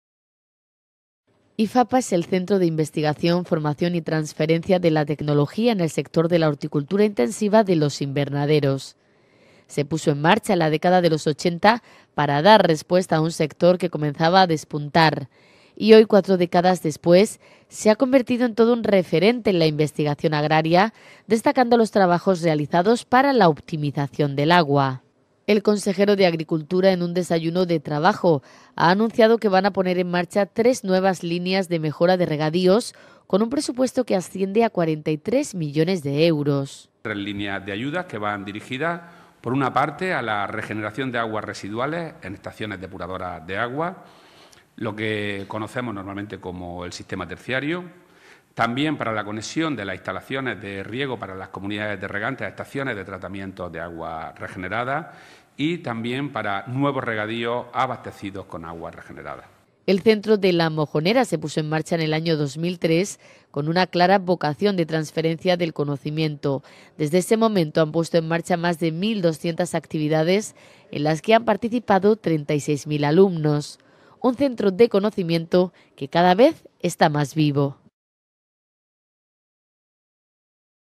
Ifapa mejorará la investigación en materia de agua
IFAPA es el centro de investigación, formación y trasnferencia de la tecnología en el sector de la horticultura intensiva de los invernaderos. Se puso en marcha en la década de los 80 para dar respuesta a un sector que comenzaba a despuntar, y hoy cuatro décadas después se ha convertido en todo un referente en la investigación agraria, destacando los trabajos realizados para la optimización del agua.El consejero de agricultura, Rodrigo Sánchez,  en un desayuno de trabajo ha anunciado que van a poner en marcha tres nuevas líneas de mejora de regadíos, con un presupuesto que asciende a 43 millones de euros.El centro de la Mojonera se puso en marcha en el año 2003 con una clara vocación de transferencia del conocimiento, desde ese momento han puesto en marcha más de 1200 actividades en las que han participado 36.000 alumnos. Un centro del conocimiento, que cada vez está más vivo.